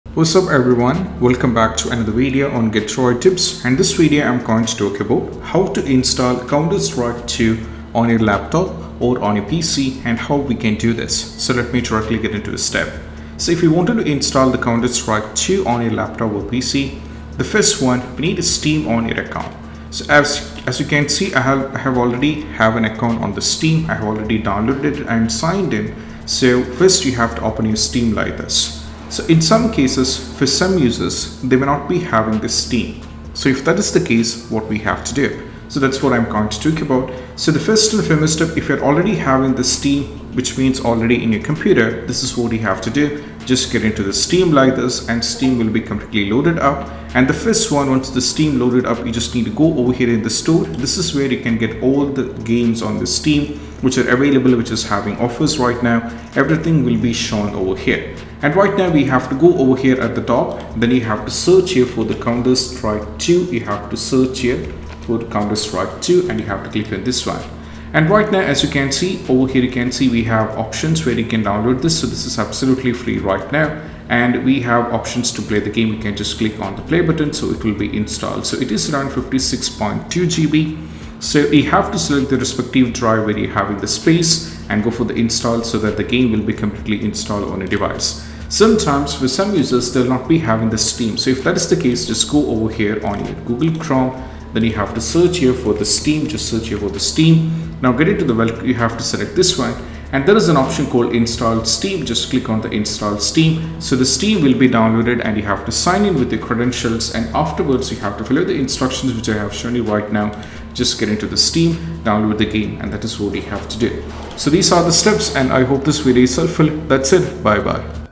How To Install Counter Strike 2 on PC and Laptop [2025]
This step-by-step guide walks you through downloading and installing Counter-Strike 2 (CS2) on a Windows laptop. You’ll learn how to install Steam, locate CS2 in the store, check system requirements, and get the game set up—perfect for both CS:GO veterans and new players alike